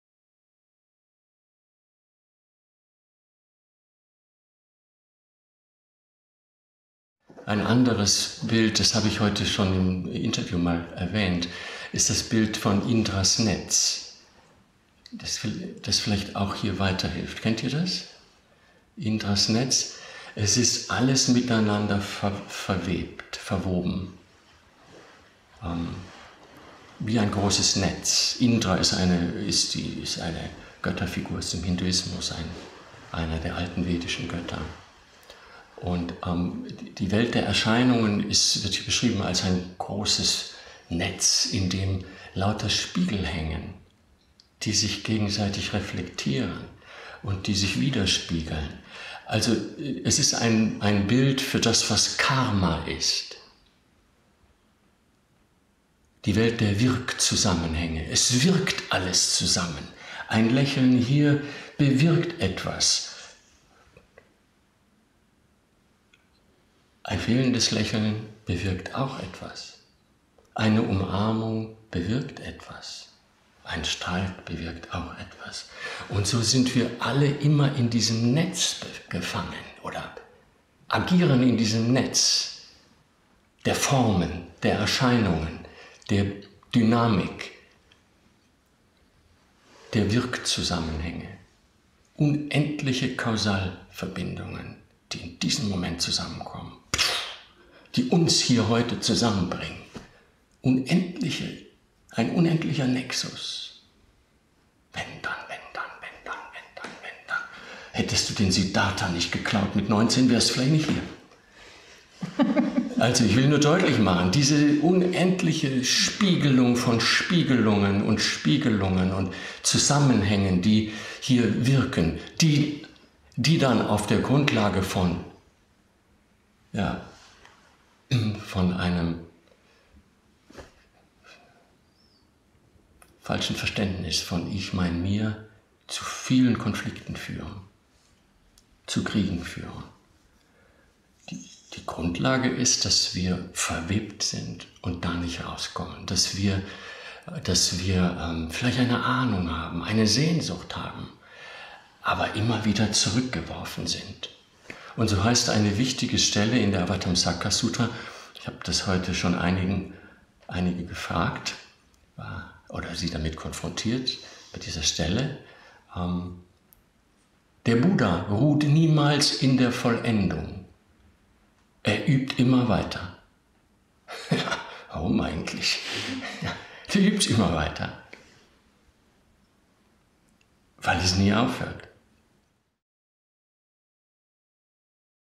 Warum übt der Buddha immer weiter?
Dharmarede von Zen-Meister Ji Kwang.